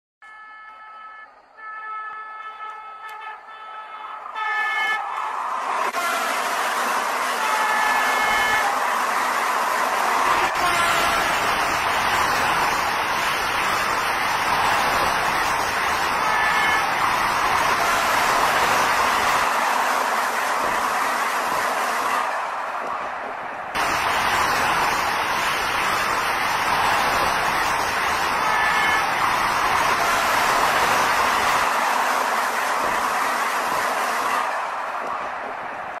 בנסיעת הבכורה: הרכבת המהירה בהודו דרסה פרה והושבתה
הודו הציגה לראווה את הרכבת המהירה הראשונה שלה, שיצאה אתמול מניו דלהי לוורנאסי. בדרך חזור היא התנגשה בפרה ונתקעה על המסילה